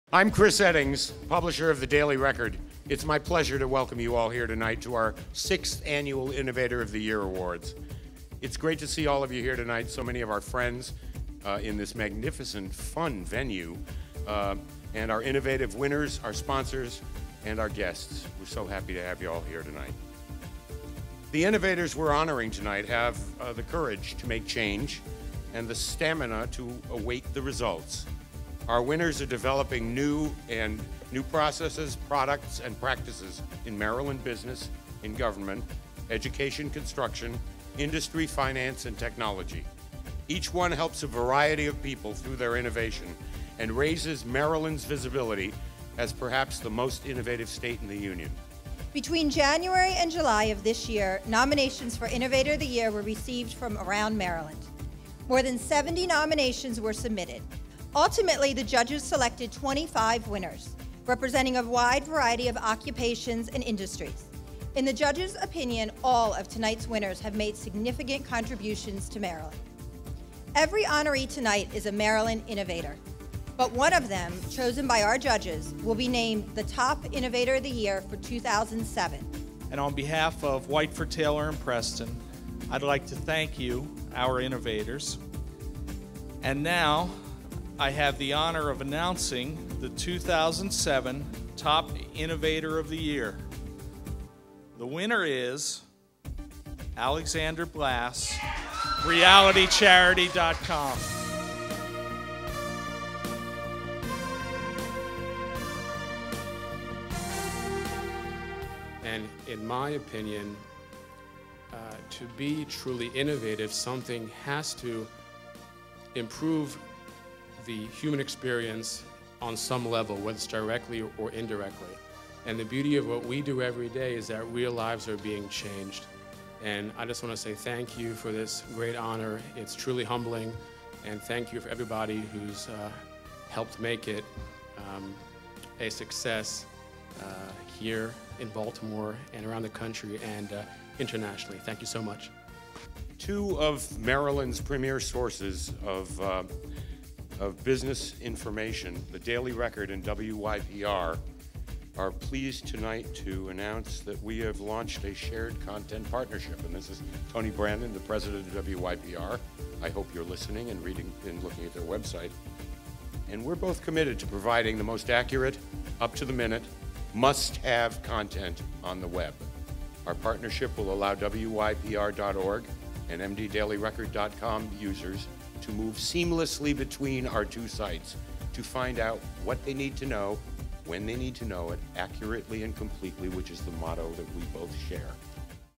Innovator of the Year Recap Video
A Recap of the 2007 Innovator of the Year program.